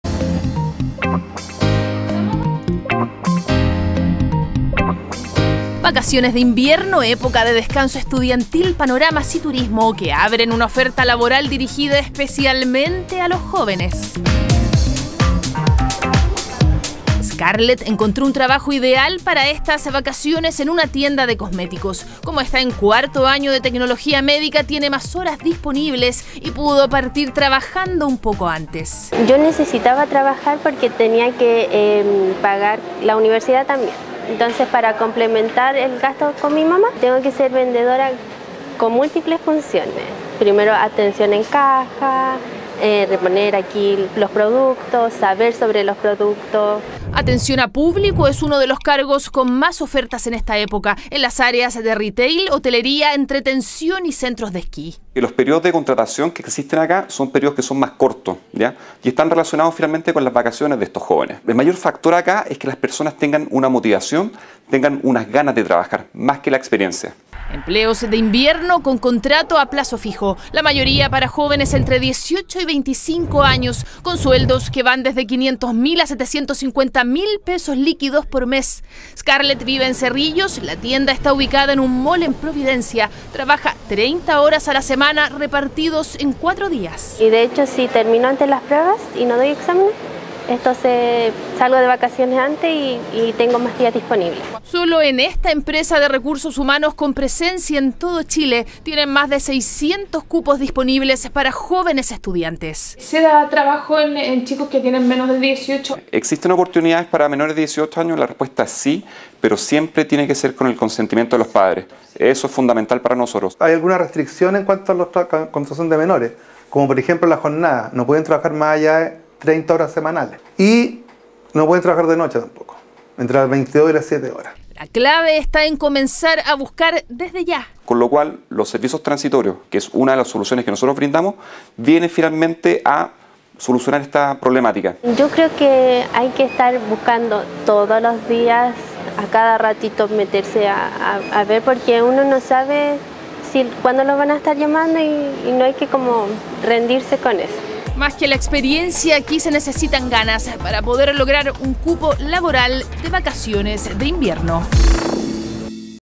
Vacaciones de invierno: Temporada de trabajos para jóvenes estudiantes
Vacaciones de invierno, época de descanso estudiantil, panoramas y turismo que abren una oferta laboral dirigida especialmente a los jóvenes.